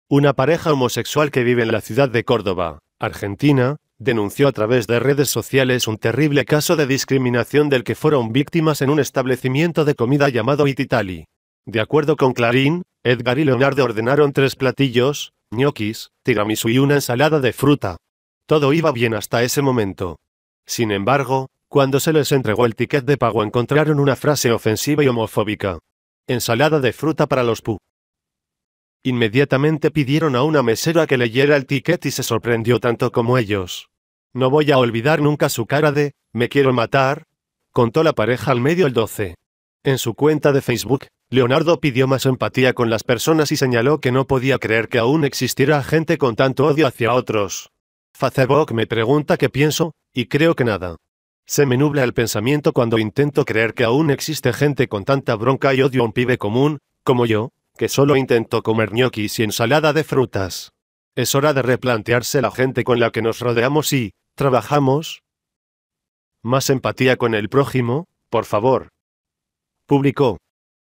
Pareja gay sale a cenar y en el ticket les dejan mensaje homofóbico
Una pareja homosexual que vive en la ciudad de Córdova, Argentina, denunció a través de redes sociales un terrible caso de discriminación del que fueron víctimas en un establecimiento de comida llamado Ititaly.

De acuerdo con Clarín, Edgar (29 años) y Leonardo (35) ordenaron tres platillos: ñoquis, tiramisú y una ensalada de fruta.

Todo iba bien hasta ese momento. Sin embargo, cuando se les entregó el ticket de pago encontraron una frase ofensiva y homofóbica.

“Ensalada de fruta para los pu**”.

Inmediatamente pidieron a una mesera que leyera el ticket y se sorprendió tanto como ellos.

No voy a olvidar nunca su cara de ‘me quiero matar’”, contó la pareja al medio El Doce.

En su cuenta de Facebook, Leonardo pidió más empatía con las personas y señaló que no podía creer que aún existiera gente con tanto odio hacia otros.

Facebook me pregunta qué pienso… y creo que nada. Se me nubla el pensamiento cuando intento creer que aún existe gente con tanta bronca y odio a un pibe común, como yo, que solo intentó comer ñoquis y ensalada de frutas. Es hora de replantearse la gente con la que nos rodeamos y ‘trabajamos’. ¡Más empatía con el prójimo, por favor!”, publicó.